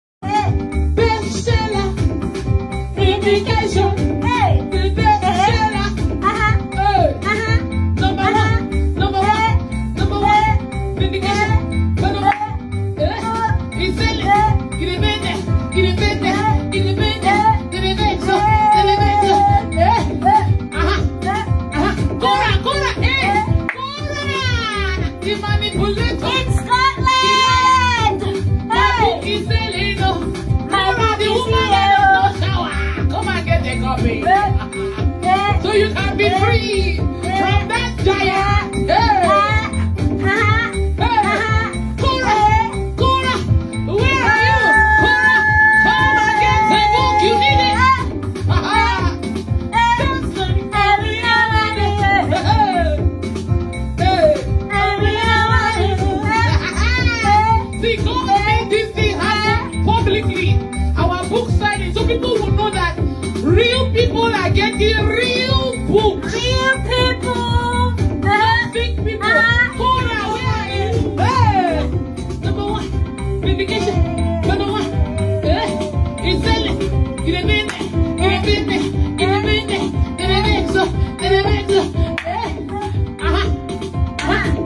People all over the world are SO EXCITED to receive their copy of VINDICATION! To God be the glory!
People all over the world are SO EXCITED to receive their copy of VINDICATION! I love this video! To God be the glory!
Get your copy at Elevivpublishing.com